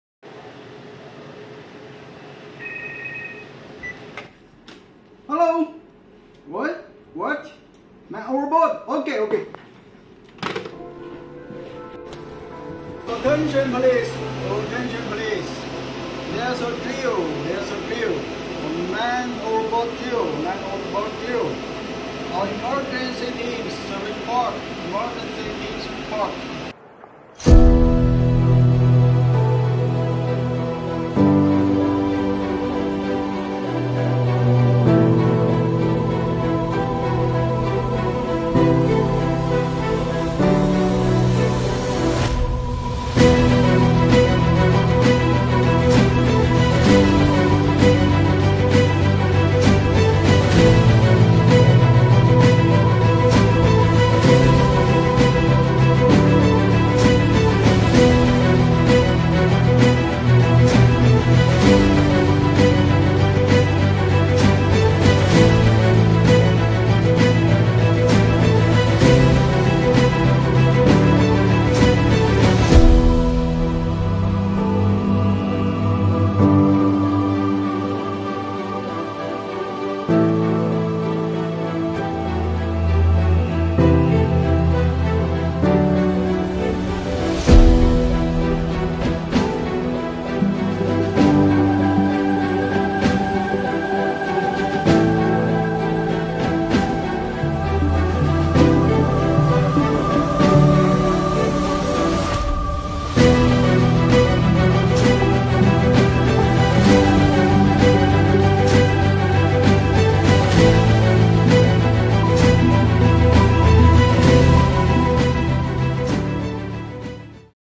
Man Overboard DRILL !! Simulated on Rig Jackup Asian Endeavour 1
This "MOB" Drill was Monthly need to do. Repeat and repeat . For re-freshing the Memories of Crew  and bring acknowledge for New members.

What kind action need to do if real situation Happen.

Simulated use a "Dummy Person" drop to the  water.

Copyright : Rafid_Argan